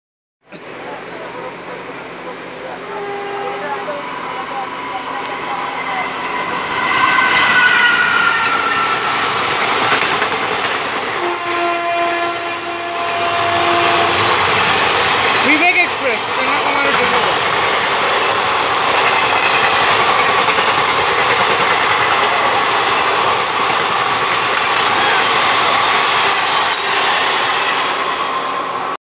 India's longest running train Dibrugargh (DBRG)Kanyakumari (KAPE) Vivek express blast kalipahari🚂
Howrah WAP4 in charge.